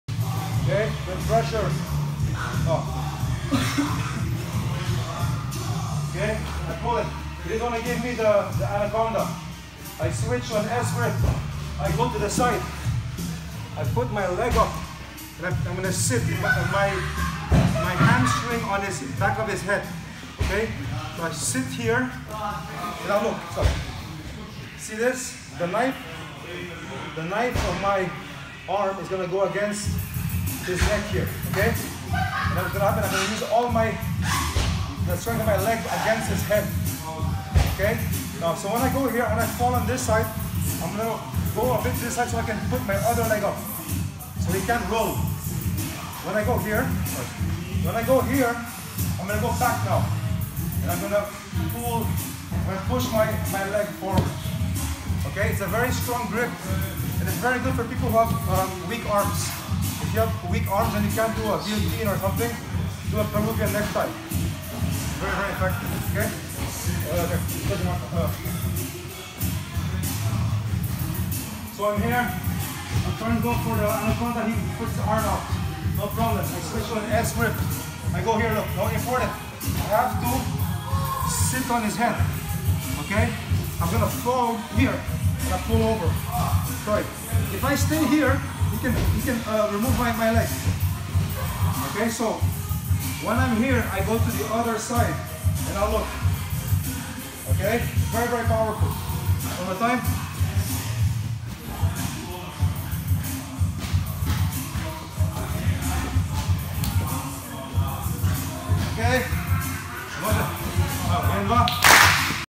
Front headlock to Peruvian necktie
BJJ black belt Gile Huni shows how to set up a very efficient Peruvian necktie from the front headlock position.